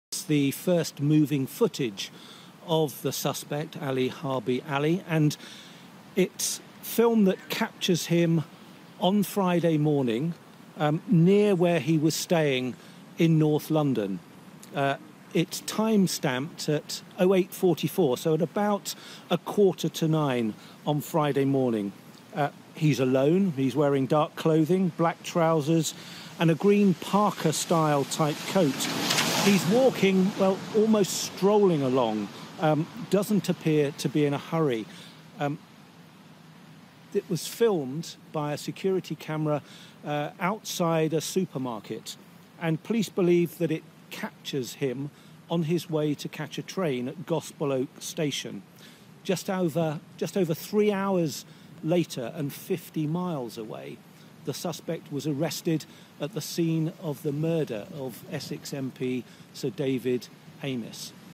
CCTV of Sir David Amess murder suspect on morning of attack
CCTV appears to show murder suspect Ali Harbi Ali hours before Conservative MP Sir David Amess was stabbed to death.

The 25-year-old was seen walking slowly with a backpack across his right shoulder and his left hand in his pocket near his North London home on Friday morning.

He is thought to have been walking to Gospel Oak train station when he was filmed from outside a convenience store on Highgate Road at 8.44am.

In the first moving footage of the murder suspect obtained by Sky News, he is seen leisurely walking down the street dressed in black trousers and a green parka-style coat.

Just over hours later, he was arrested for the murder of the Conservative MP and father-of-five, Sir David.

Police are investigating Ali’s movements in the hours leading up to the attack and are probing whether he boarded a train at the nearby station to travel to Leigh-on-Sea, Essex.

The shop manager who captured the footage said cops had asked to see the CCTV on Saturday morning before he handed over a copy of the video.

He said they specifically wanted to watch between a timeframe of 08.30am and 9am.

Continue reading
Moment Sir David Amess murder suspect seen walking to train station on morning of attack that killed MP